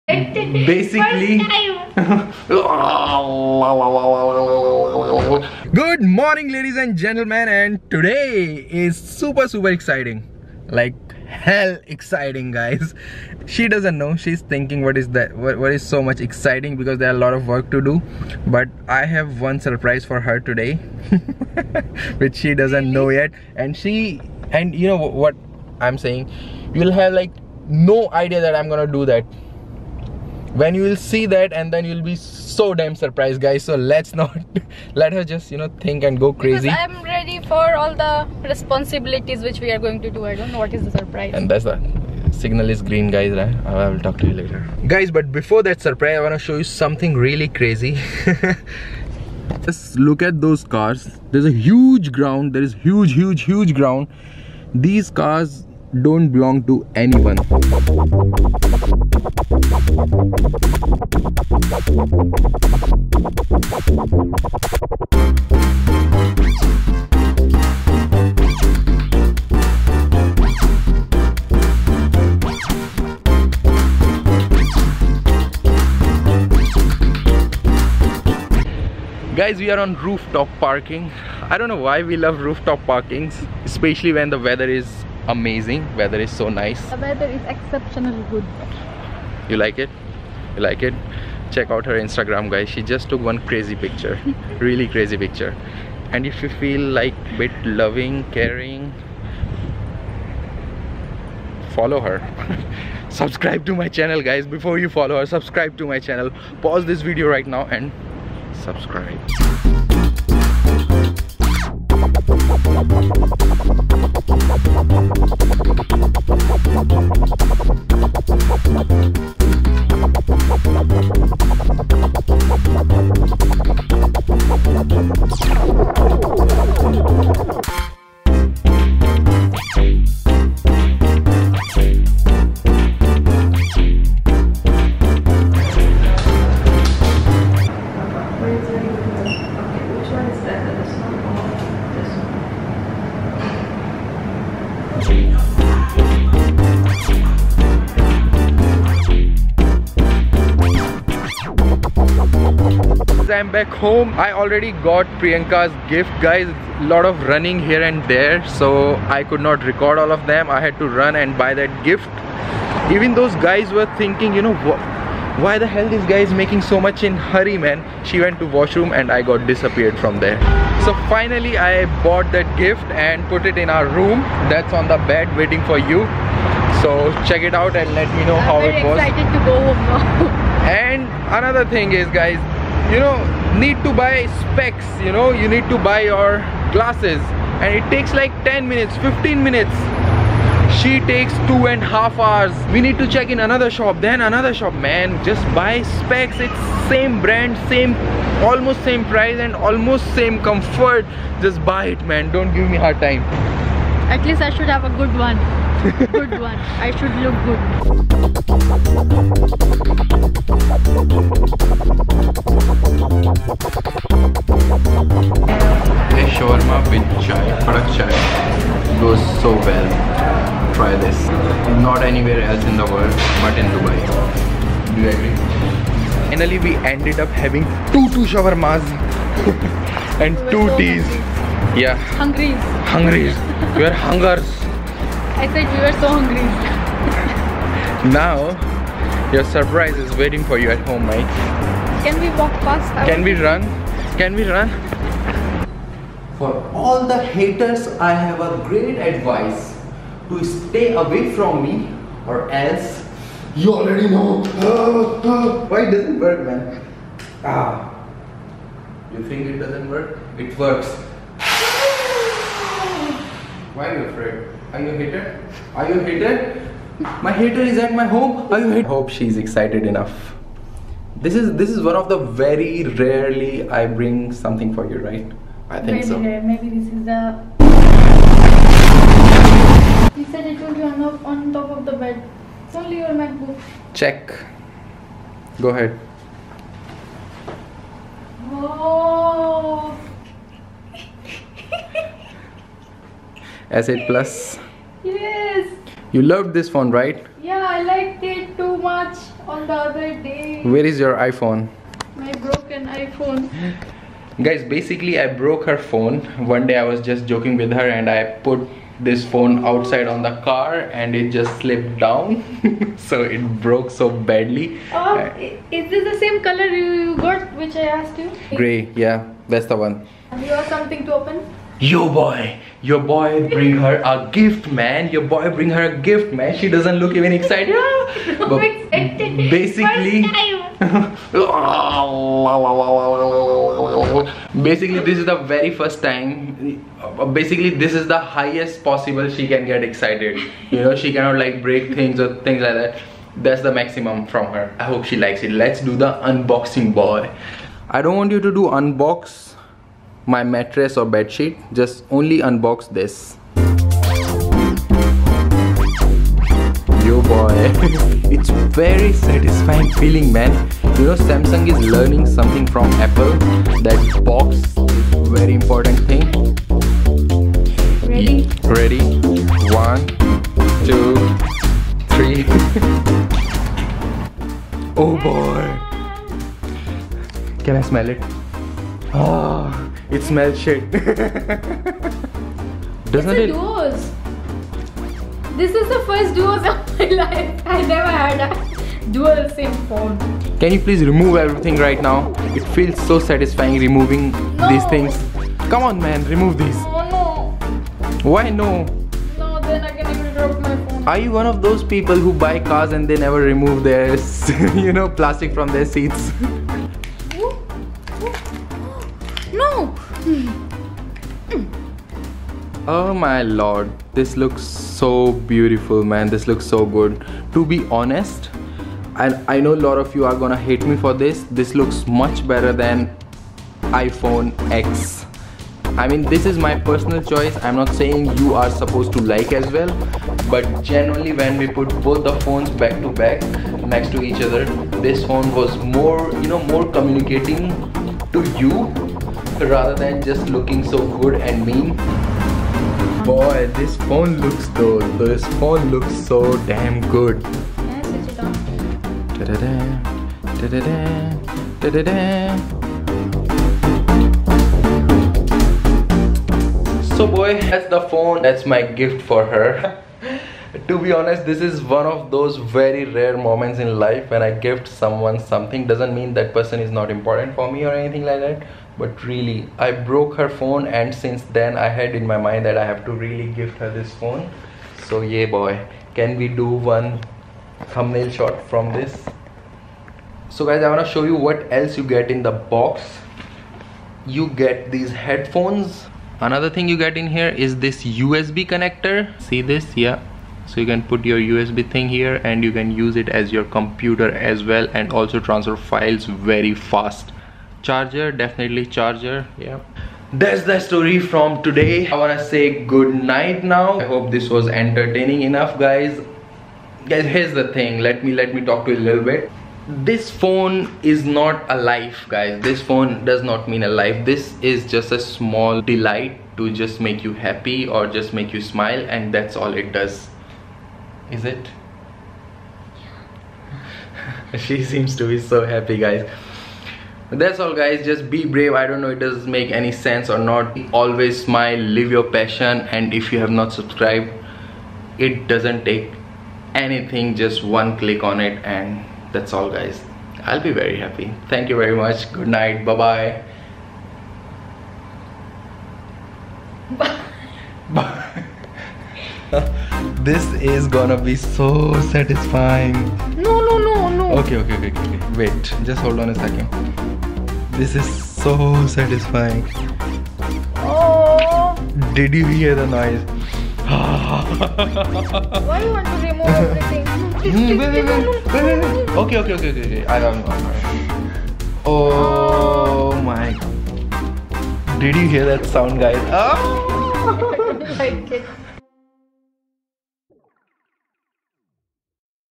Surprising her with Samsung S8+ over IPhone X
An amazing day, with great drive way. Best time spent while shopping the Samsung Galaxy S8+. As it was long planned to gift her favorite phone after breaking her iPhone, look at the excitement on her face and the best unboxing ever...